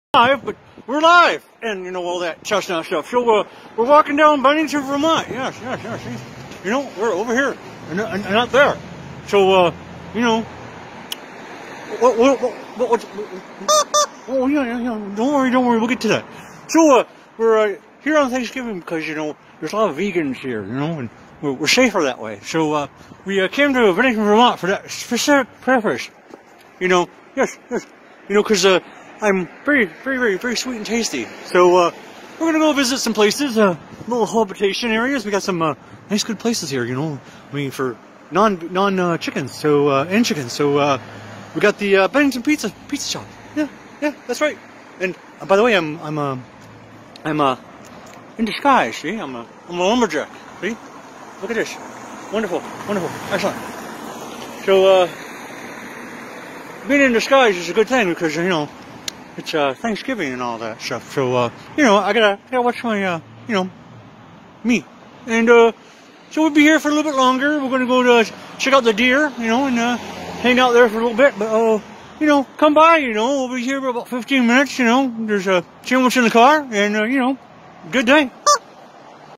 Bennington Vt on Thanksgiving
We travel to Bennington, Vt for Thanksgiving, what could possibly go wrong?